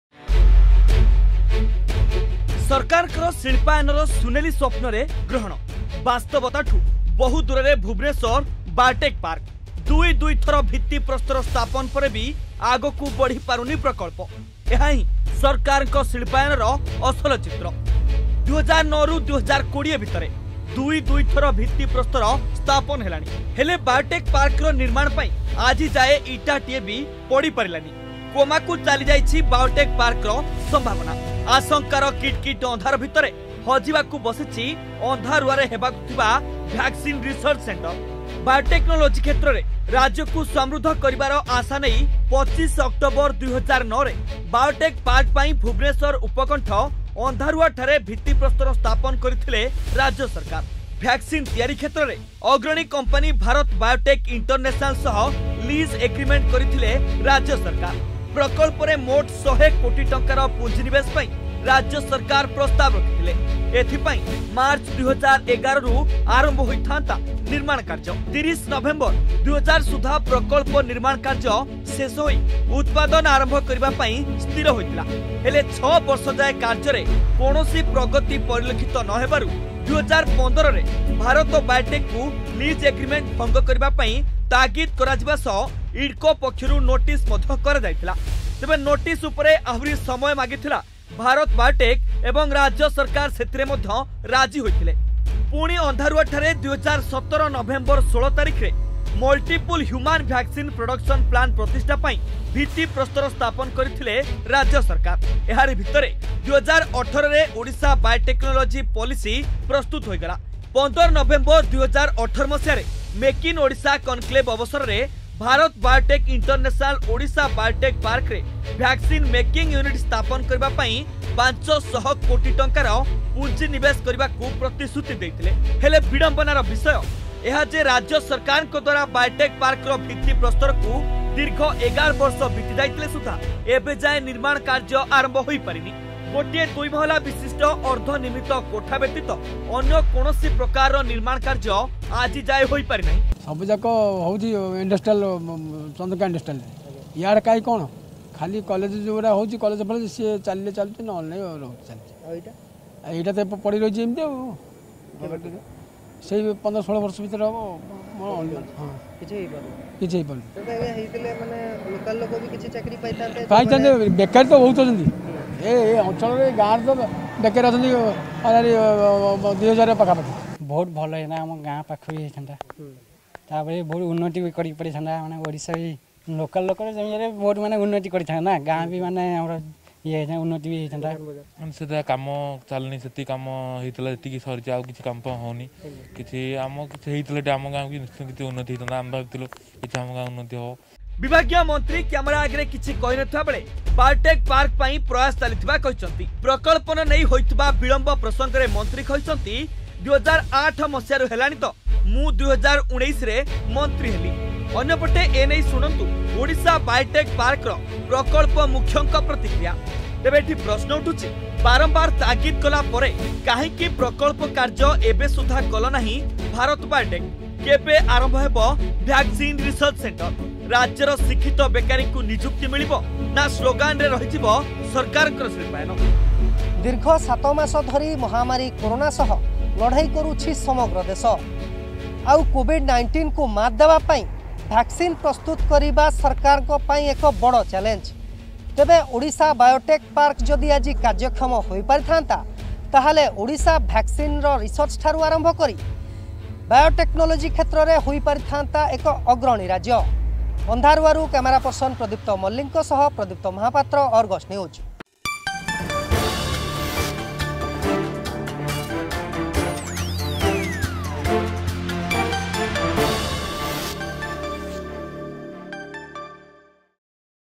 ବାସ୍ତବତାଠୁ ବହୁ ଦୂରରେ ଭୁବନେଶ୍ବରର ବାୟୋଟେକ ପାର୍କ | Bhubaneswar Biotech Park
ଅନ୍ଧାରରେ ହଜିଯିବାକୁ ବସିଛି ଅନ୍ଧାରୁଆର ଭ୍ୟାକେସିନ୍ ରିସର୍ଚ୍ଚ ସେଣ୍ଟର । 11 ବର୍ଷ ପରେ ବି ପ୍ରକଳ୍ପ ନେଇ ଇଟାଟିଏ ପଡିନାହିଁ । 500 କୋଟିର ଏହି ପ୍ରକଳ୍ପର ଭବିଷ୍ୟତ ନେଇ ସୃଷ୍ଟି ହୋଇଛି ସନ୍ଦେହ । ଯାହା ସରକାରଙ୍କ ଶିଳ୍ପାୟନ ଆଭିମୁଖ୍ୟ ଉପରେ ପ୍ରଶ୍ନବାଚୀ ସୃଷ୍ଟି କରିଛି .